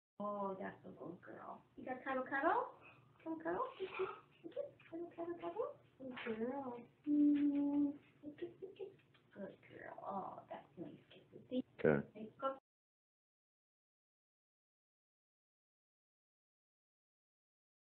Cuddle-Cuddle Kiss-Kiss SUPERCUTE Baby Scarlet Macaw
Scarlet Macaw Parrot - She is Mexican and 8 months old. She is in training for being gentle giving cuddles and kisses. When you first get your macaw baby you should handle them often but it doesn't have to be for long. I pick her up every time I walk by her perch, we do a cuddle-cuddle kiss-kiss and she goes back. Now she is happy playing by herself and also with me. She does step up, but also knows that when I pick her up it is not just to go to the cage. We walk around the house for a little bit and she is happy to return to the perch. The most important thing is to develop trust with your parrot and she will grow into a friendly and well balanced bird.
Sorry the vid is low quality, it was from a cell phone even the sound is a little wonky. If I can fix it i will upload a new one.